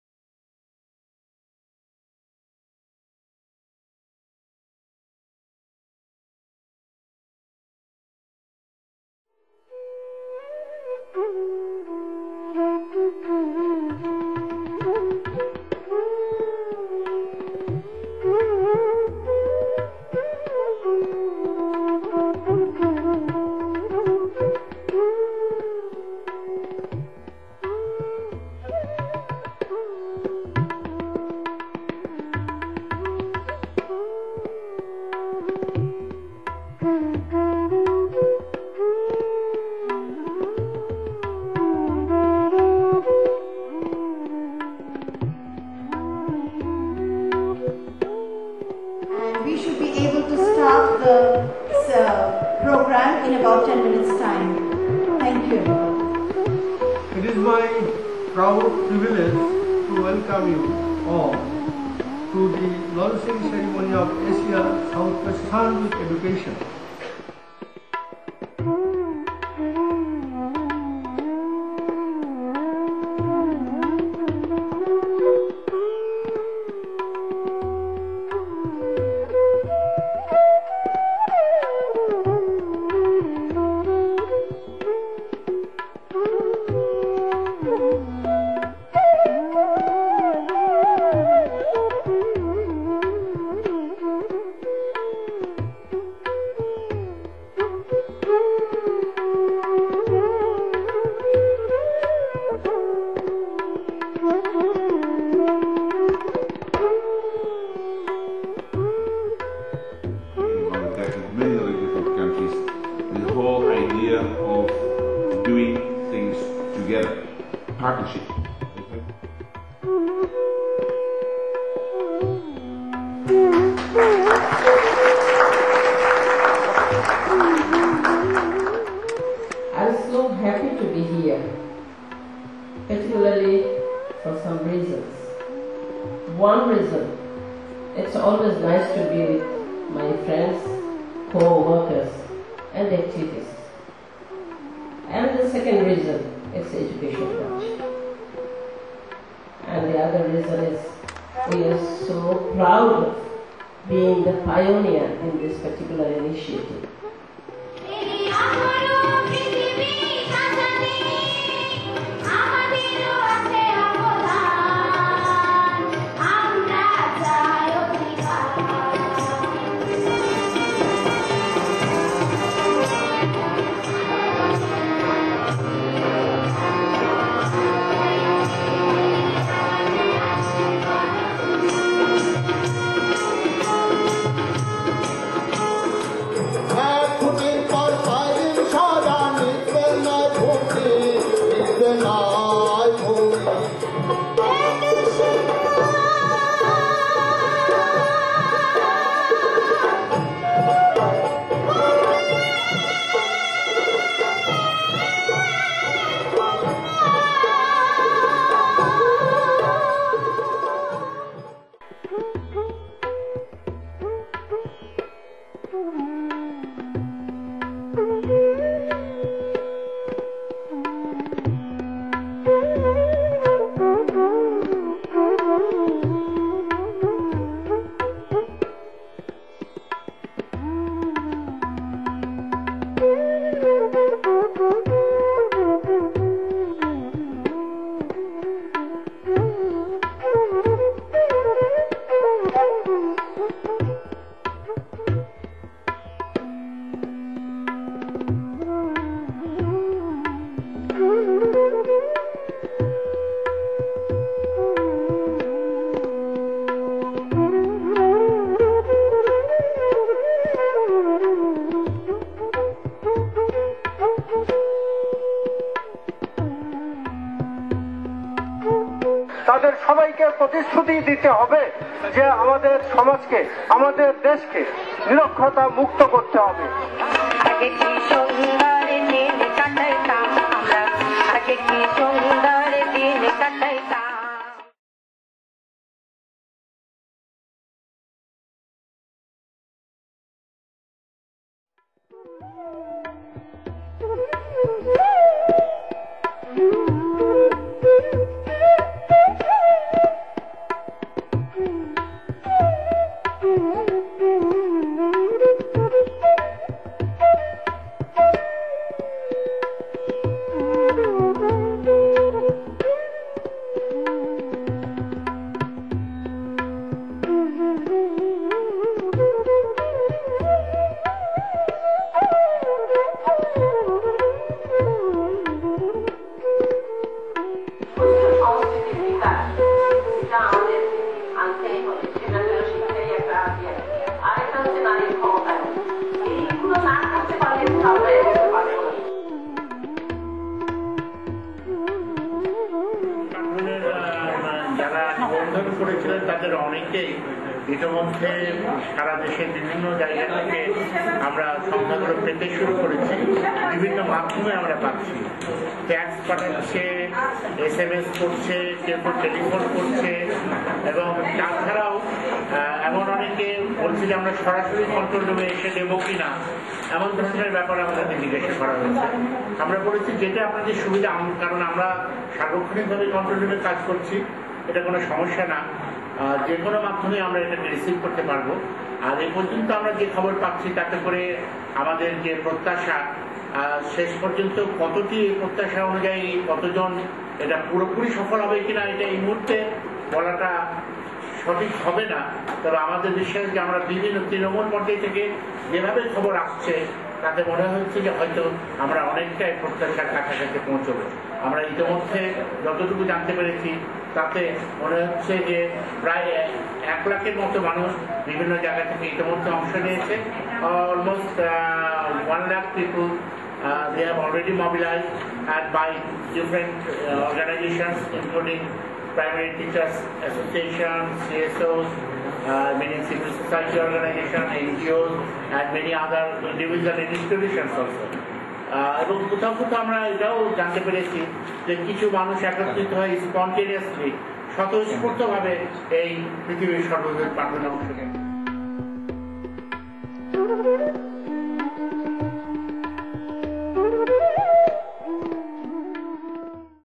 World's Biggest Lesson in Bangladesh
Short film showing the amazing achievements of holding the World's Biggest Lesson in Bangladesh organized by CAMPE